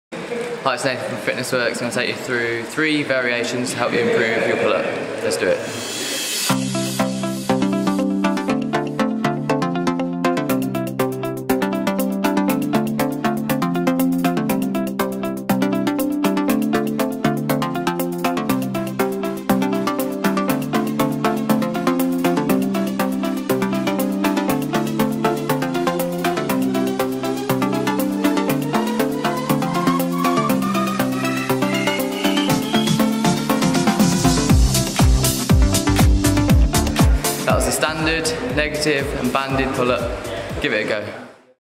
PULL UP VARIATIONS - with FWX Coach Nathan
Are you struggling to do a pull up or do you need new pull up idea's to increase your strength?

FWX Coach Nathan take's you through variations to add into your workout!